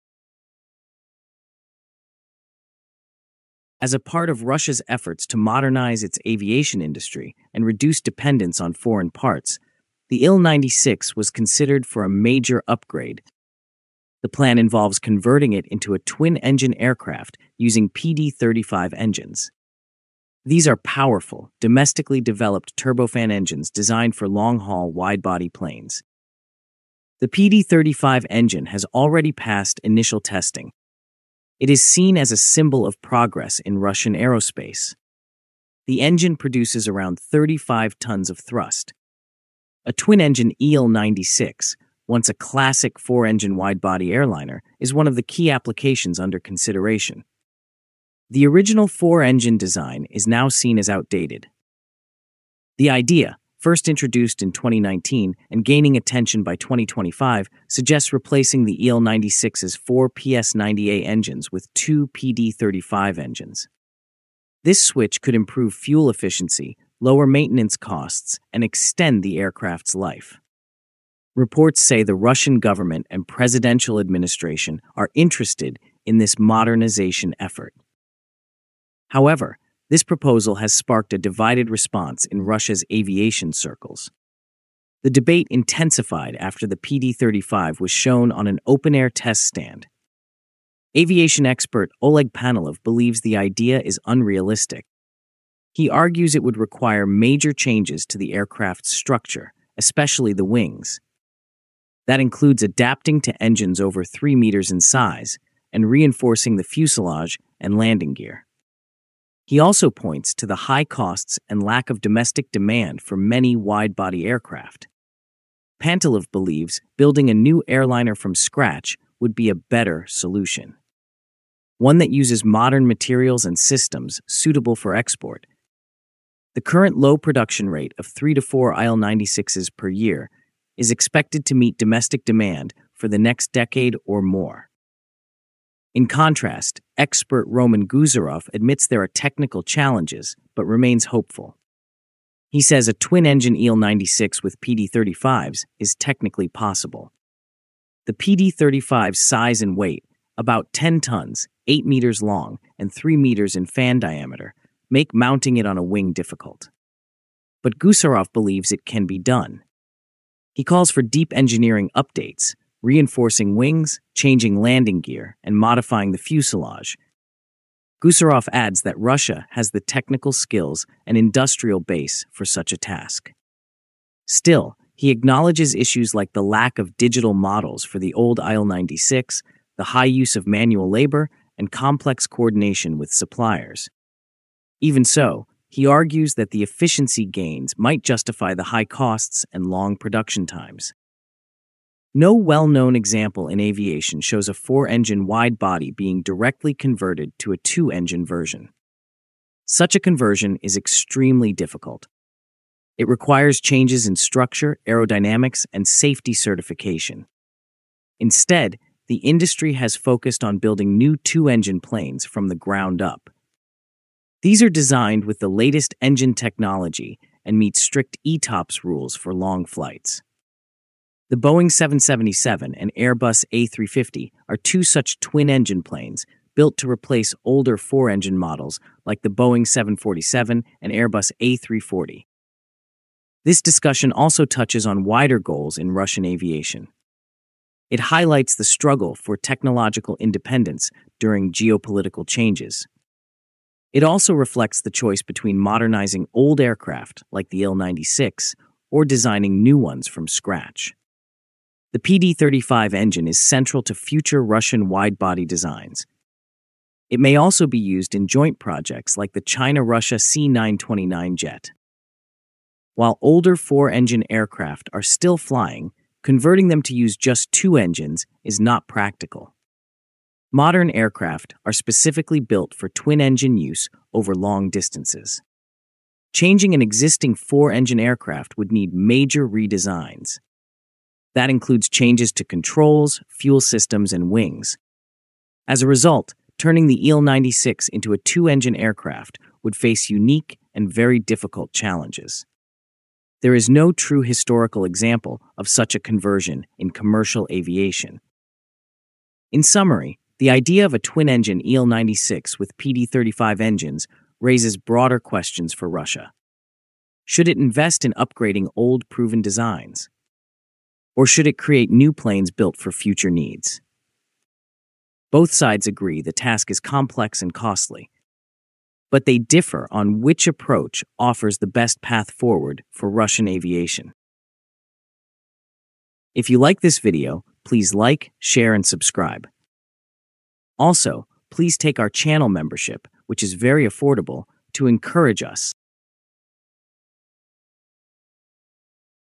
Russia’s Il-96 Jet May Get Just Two PD-35 Engines—But Is It Even Possible?
Russia is planning a world-first: converting its four-engine Il-96 wide-body jet into a twin-engine aircraft using the powerful new PD-35 turbofan. While this promises better fuel efficiency and strategic independence, the aviation world is split. Some experts say it’s visionary, others call it unfeasible. In this video, we break down the controversy, the engineering challenges, and the bigger picture of Russian aviation goals.

🔧 What makes the PD-35 engine so special?
🛠️ Why has no one done a 4-to-2 engine conversion before?
🇷🇺 Could this spark a new era in Russian aerospace?